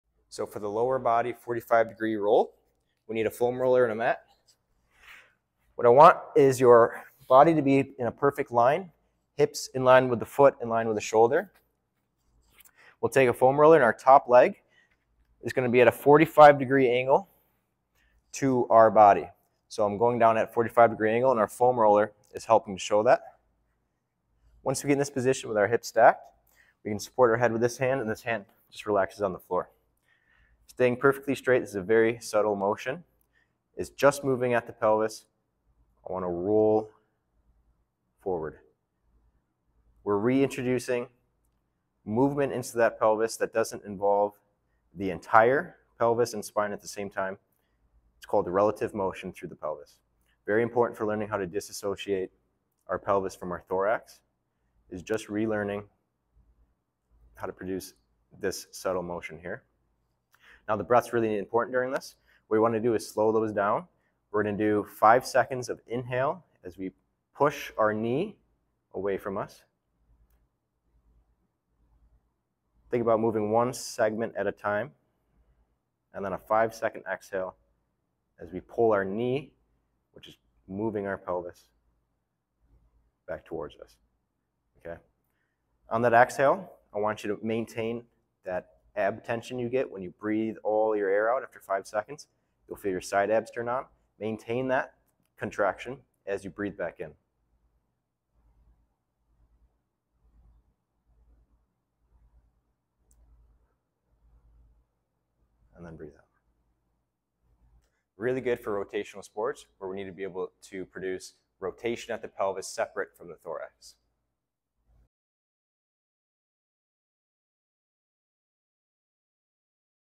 45 deg lower body half roll
This is a great drill to begin learning how to dissociate the pelvis from the upper body and gaining mobility in the pelvis.